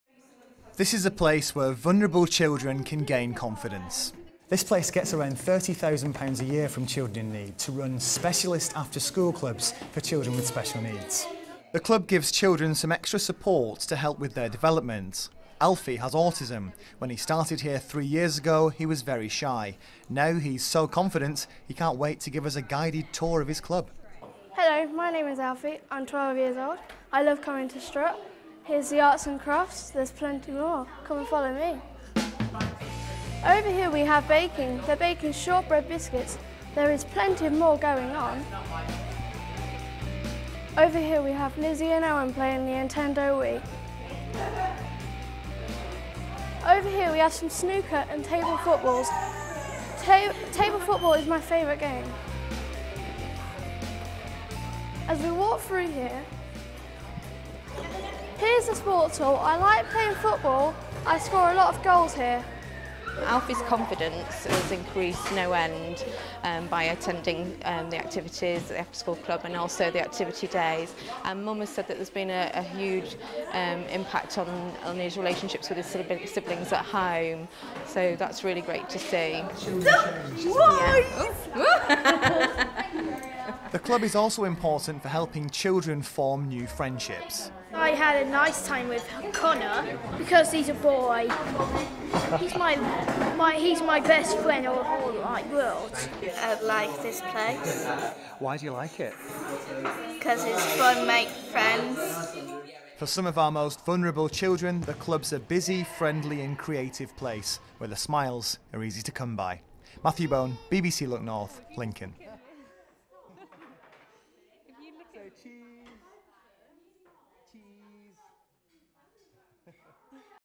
Children in Need visit KIDS Strut After School Club
Children in Need recently visited an after school club in Lincolnshire ( which they are funding until mid 2019)  to see how the project is run. They received a personal tour from service user Alfie and also spoke to the project coordinator, Claire White.